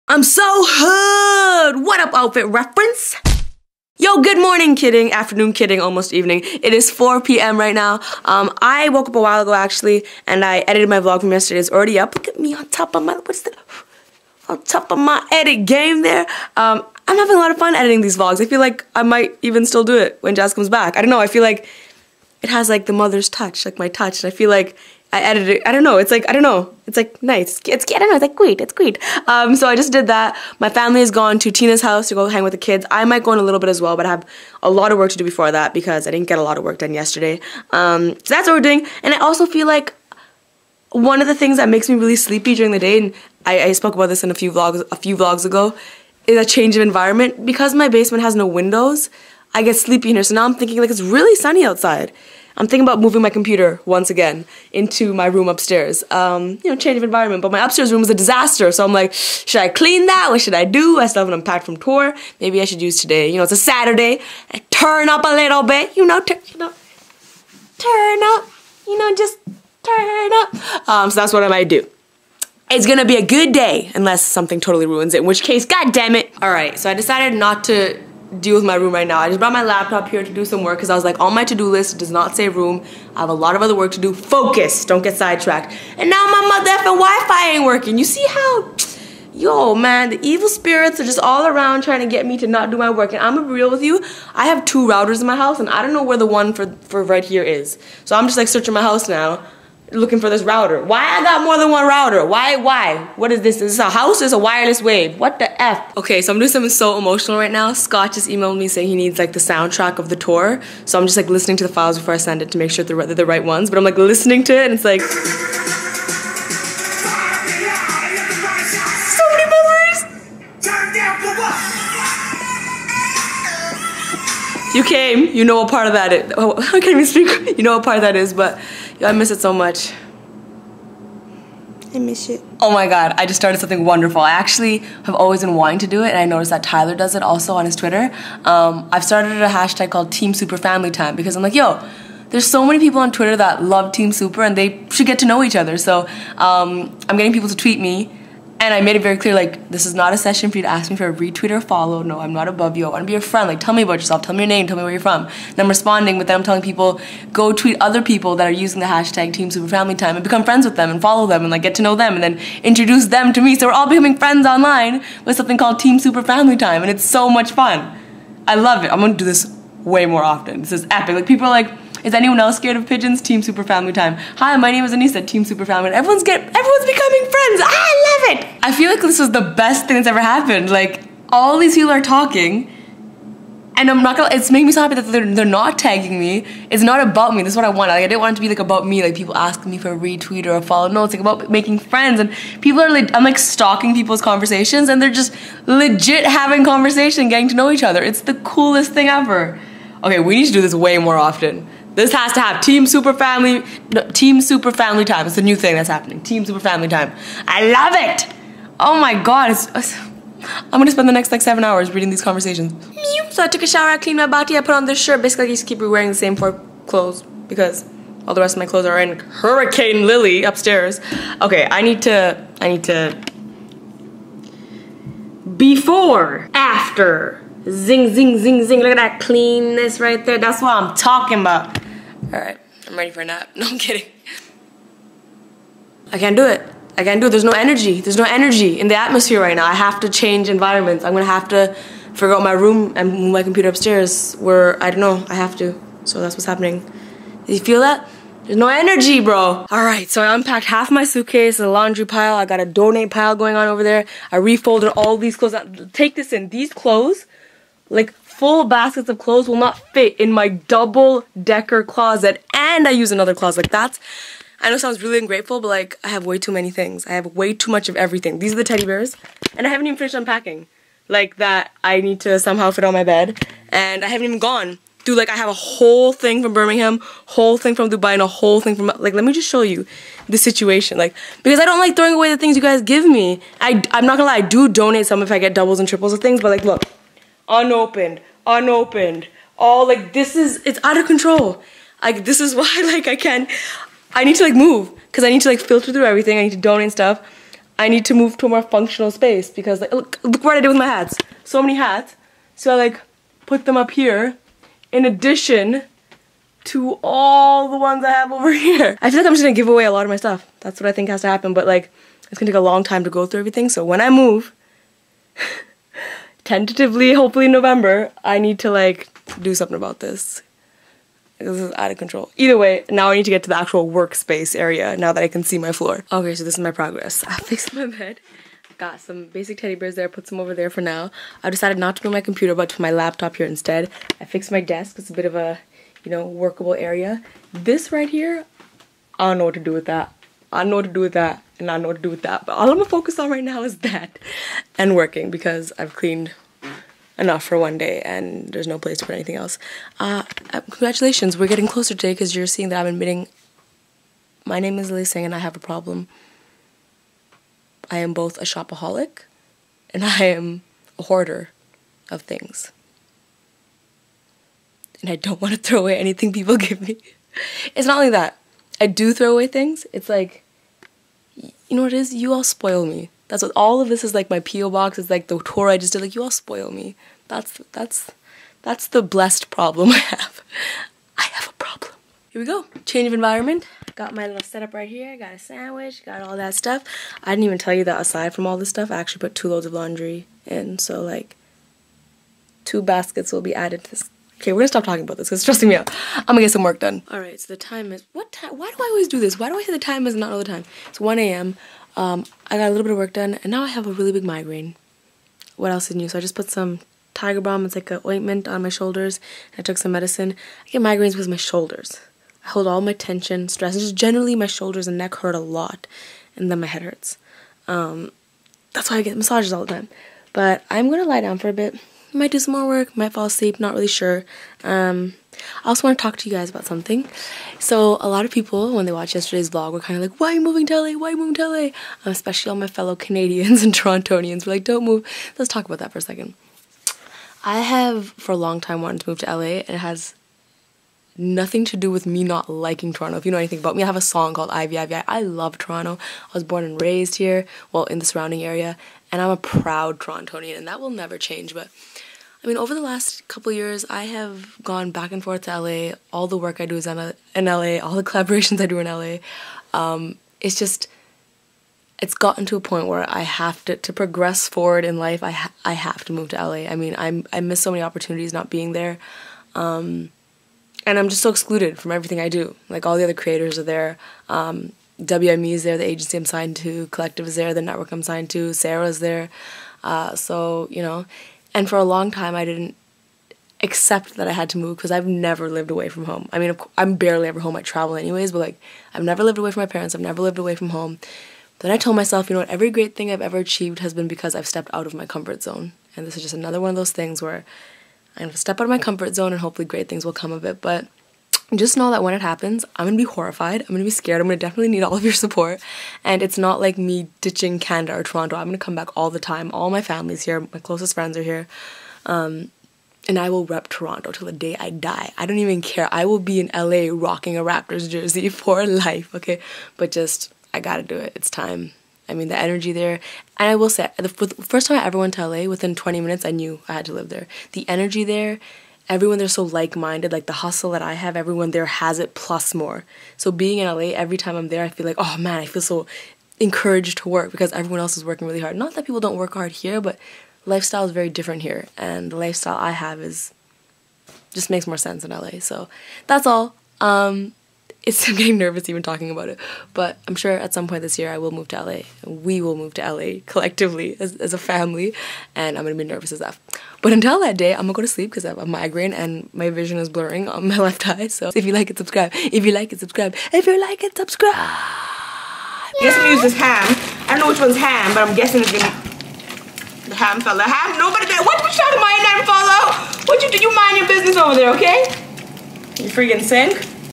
The Time I Admit I Have a Problem (Day 288)
Today I spent quality time online and made so many new friends through TeamSuperFamilyTime, went through a hurricane, admitted I have a serious problem and told you about my plans to move to LA. It's a RIDE. Enjoy!

Markham, ON L3R 6E0
Canada